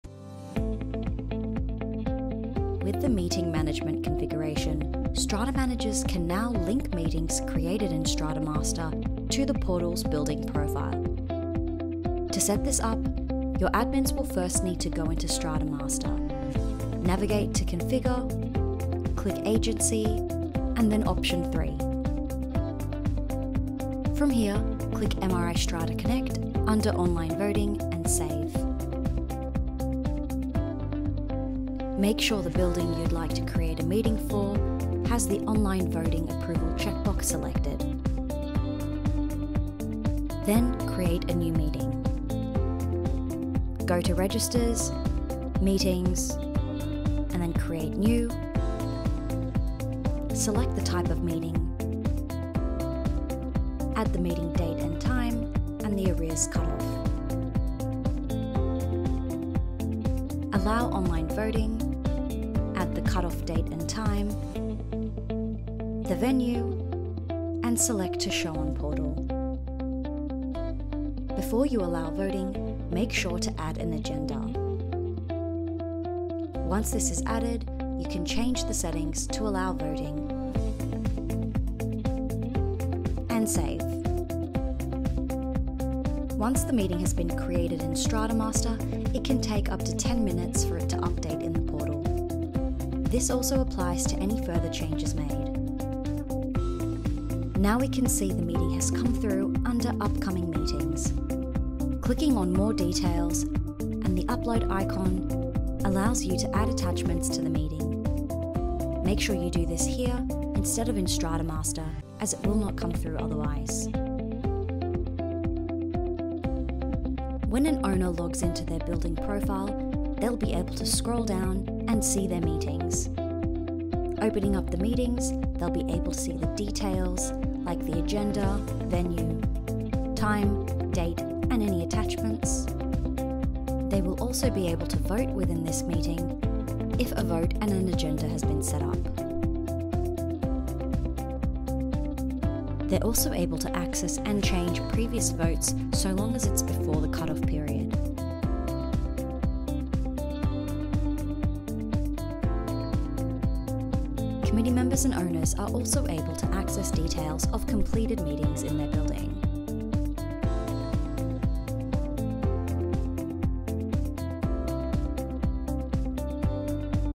Strata Master Top Tip - Strata Connect Meeting Management
Learn how to set up the Meeting Management Configuration. This helps Strata Managers link meetings created in Strata Master to the portals building profile.